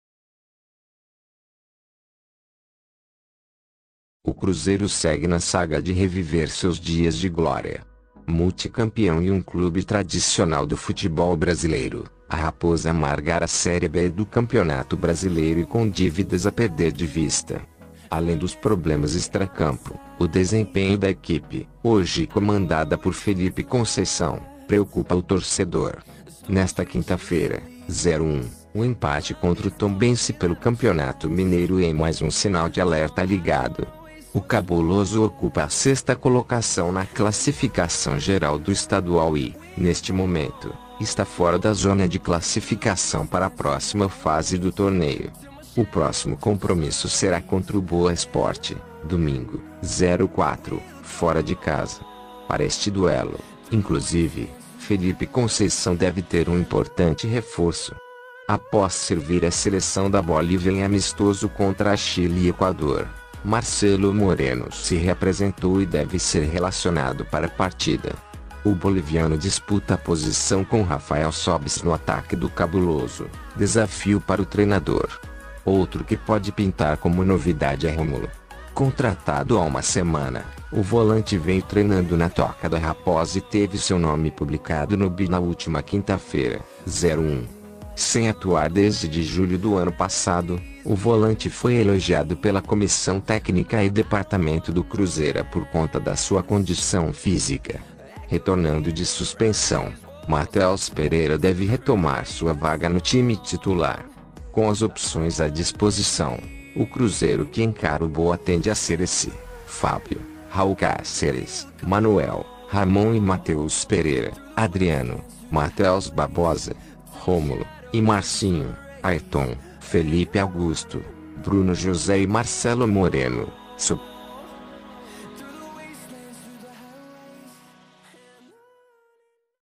BOMBA!!! NOVO REFORÇO CHEGANDO AO CRUZEIRO!!!
O Cruzeiro segue na saga de reviver seus dias de glória. Multicampeão e um clube tradicional do futebol brasileiro, a Raposa amarga a série B do Campeonato Brasileiro e com dívidas a perder de vista. Além dos problemas extracampo, o desempenho da equipe, hoje comandada por Felipe Conceição, preocupa o torcedor.

Nesta quinta-feira (01), o empate contra o Tombense pelo Campeonato Mineiro é mais um sinal de alerta ligado. O Cabuloso ocupa a sexta colocação na classificação geral do Estadual e, neste momento, está fora da zona de classificação para a próxima fase do torneio. O próximo compromisso será contra o Boa Esporte, domingo (04), fora de casa.

Para este duelo, inclusive, Felipe Conceição deve ter um importante reforço. Após servir a seleção da Bolívia em amistoso contra Chile e Equador, Marcelo Moreno se reapresentou e deve ser relacionado para a partida. O boliviano disputa posição com Rafael Sóbis no ataque do Cabuloso - desafio para o treinador.

Outro que pode pintar como novidade é Rômulo. Contratado há uma semana, o volante vem treinando na Toca da Raposa e teve seu nome publicado no BID na última quinta-feira (01). Sem atuar desde de julho do ano passado, o volante foi elogiado pela comissão técnica e departamento do Cruzeiro por conta da sua condição física.

Retornando de suspensão, Matheus Peireira deve retomar sua vaga no time titular. Com as opções à disposição, o Cruzeiro que encara o Boa tende a ser esse: Fábio; Raúl Cáceres, Manoel, Ramon e Matheus Pereira; Adriano, Matheus Barbosa (Rômulo) e Marcinho; Airton (Felipe Augusto), Bruno José e Marcelo Moreno (Sobis).

COMENTE!